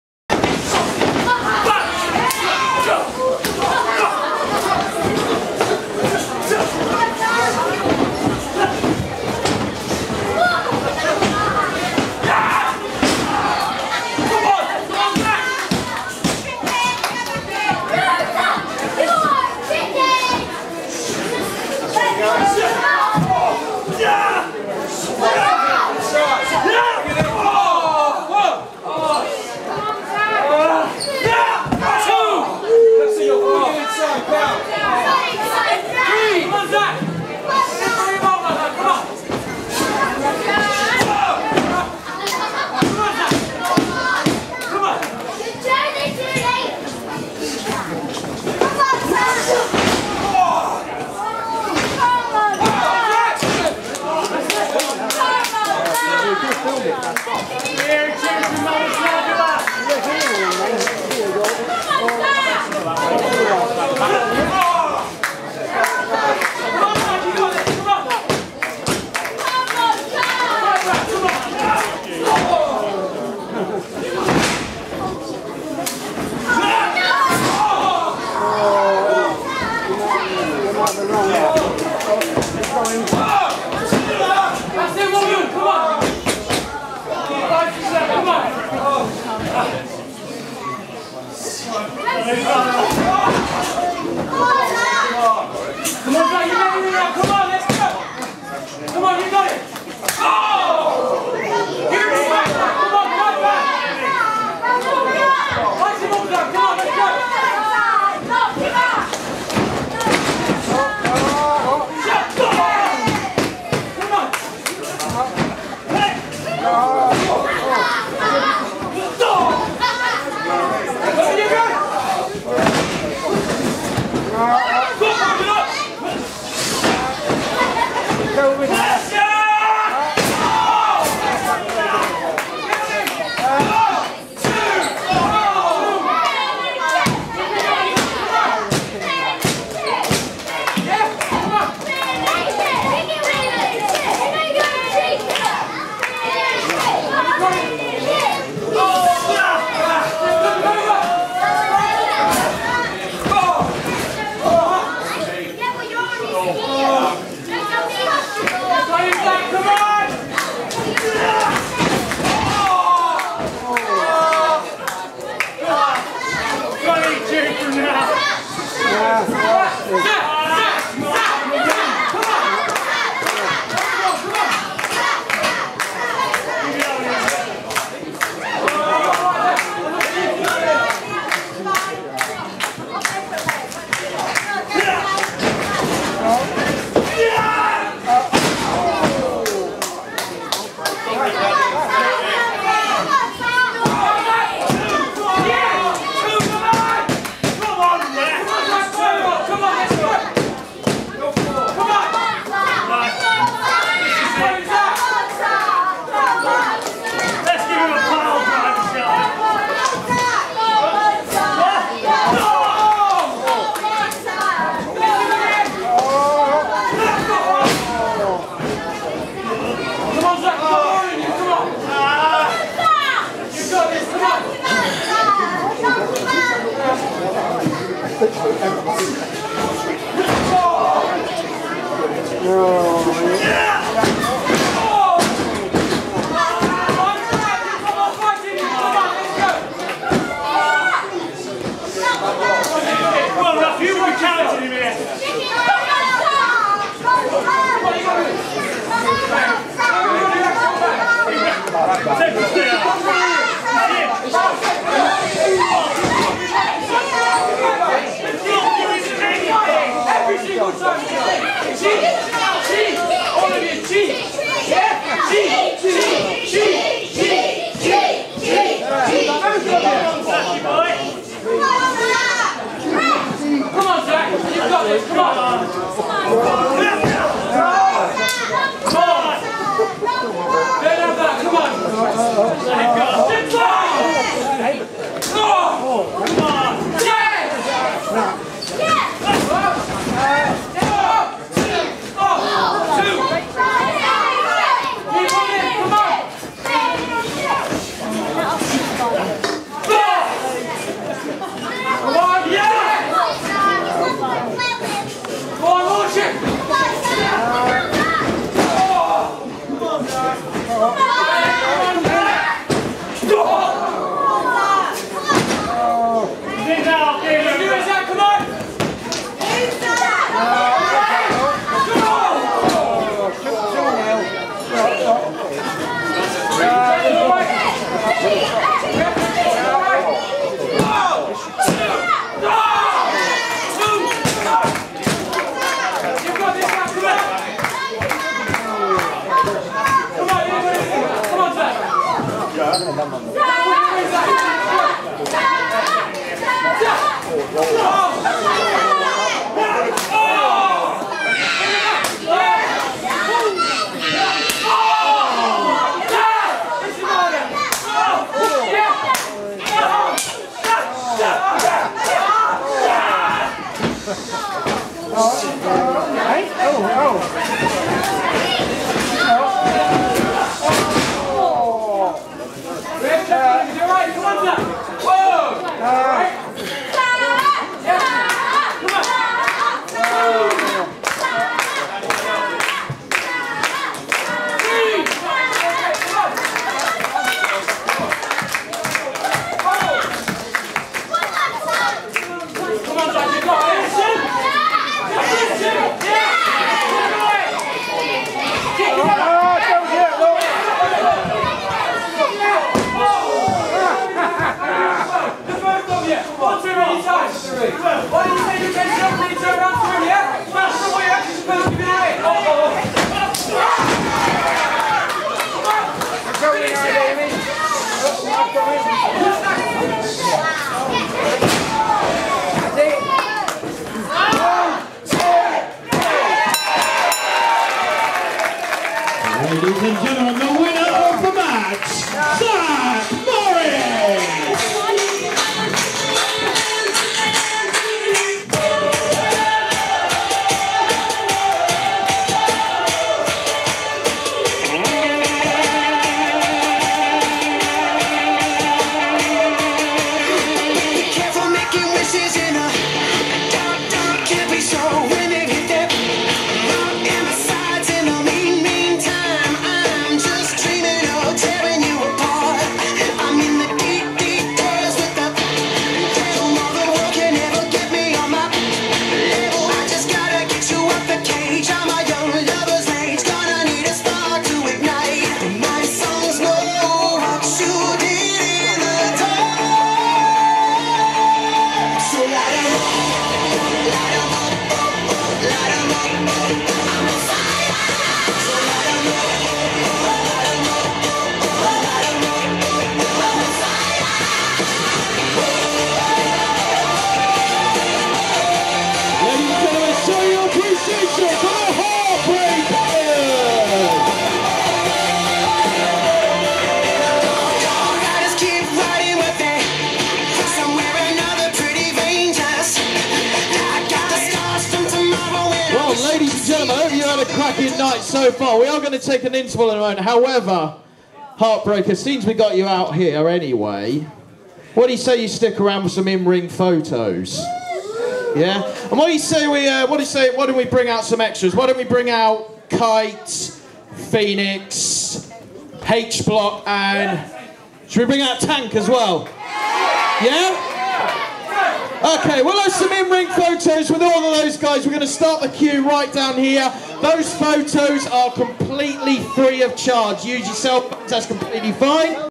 Dani Graves vs Zach Morris - House of Pain Wrestling - 26/04/2019
'Cannonball' Dani Graves vs Zach Morris - House of Pain Wrestling - 26/04/2019

Rainworth Working Men's Club, Rainworth, Nottinghamshire.